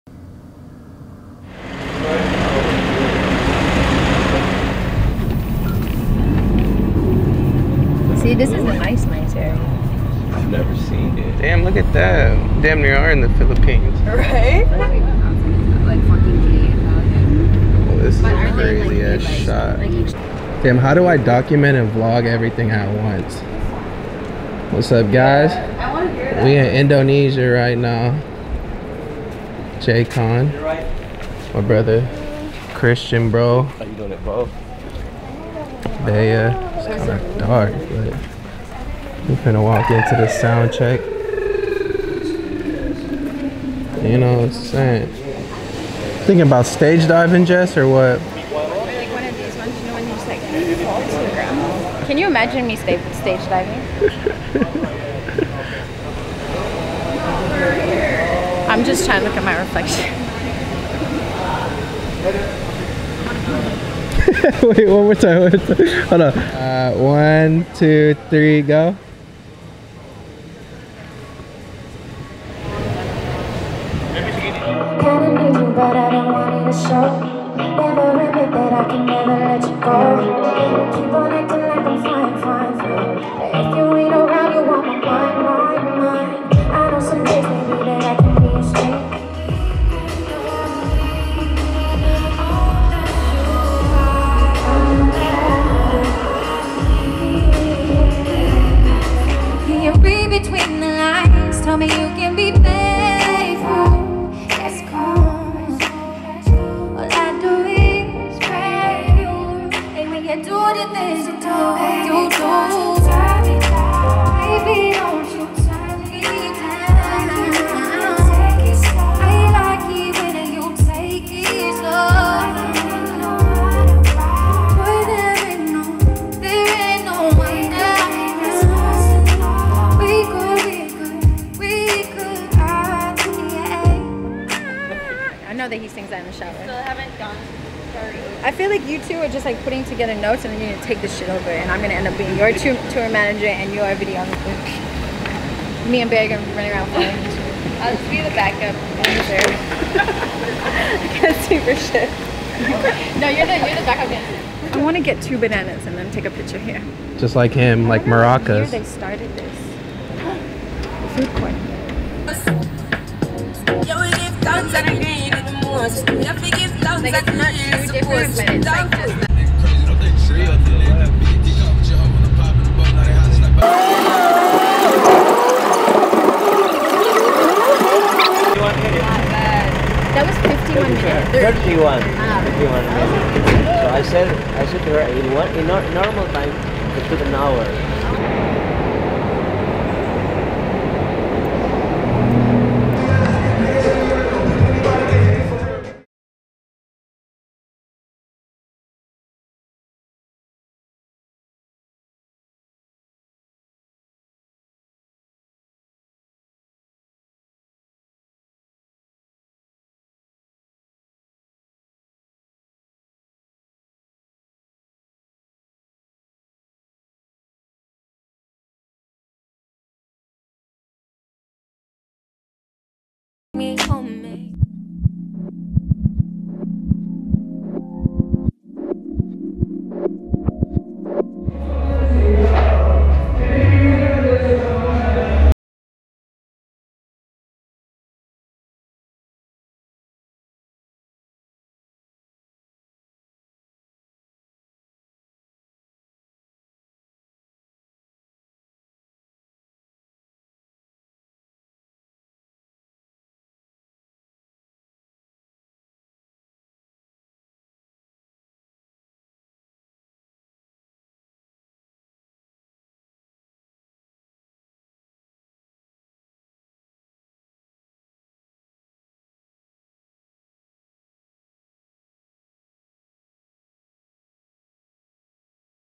Jess Connelly // WE THE FEST 2019 VLOG
Day in the life of Jess Connelly!
Sorry guys, forgot to UNMUTE the audio for the last part of video.
Check out @jessconnelly page on IG for the recaps!